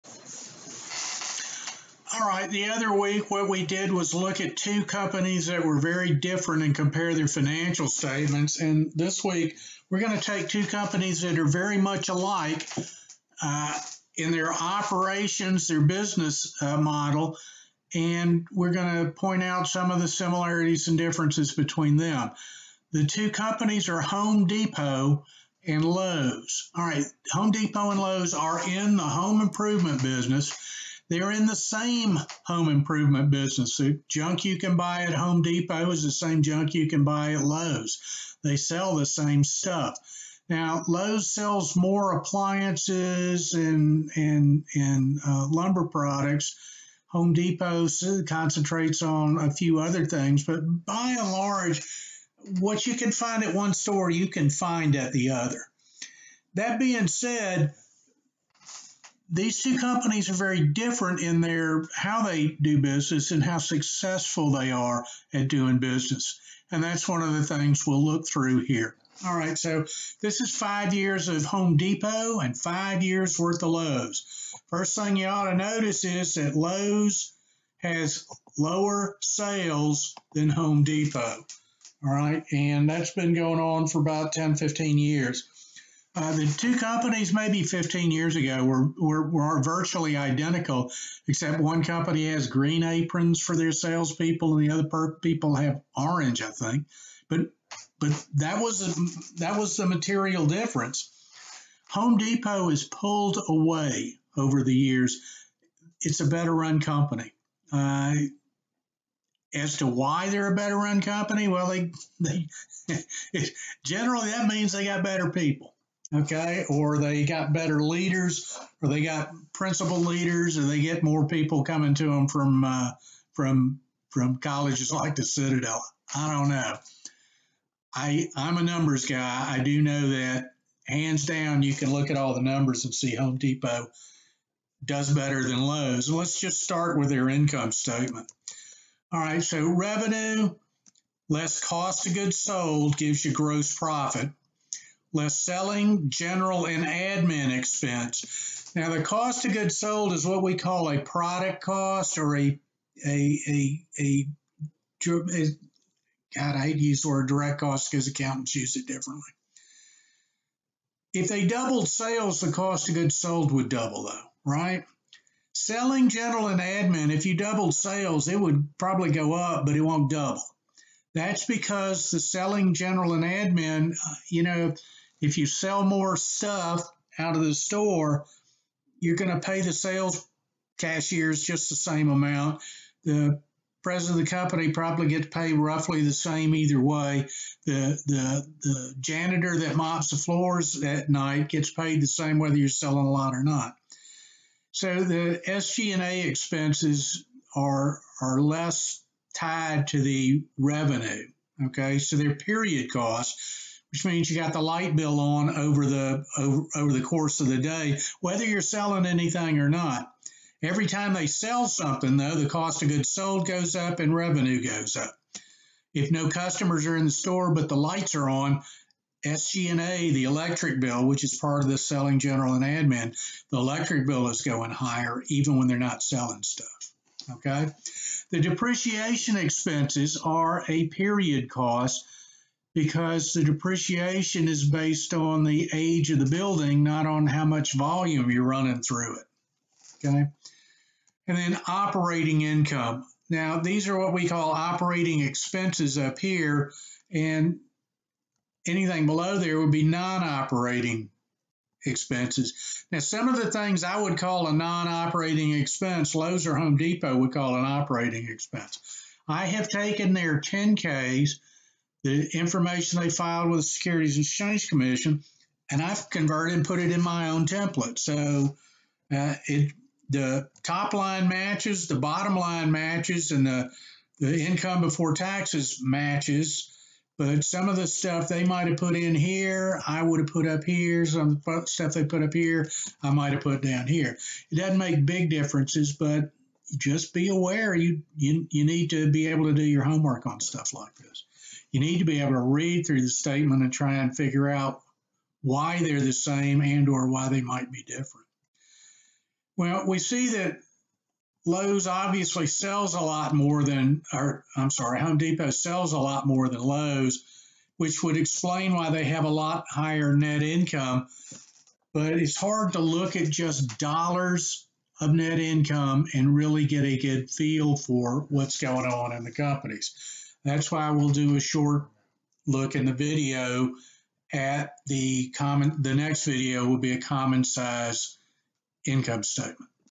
Compare HD and LOW Income Statements
Five years of historical income state data, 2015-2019, for Home Depot (HD) and Lowes (LOW)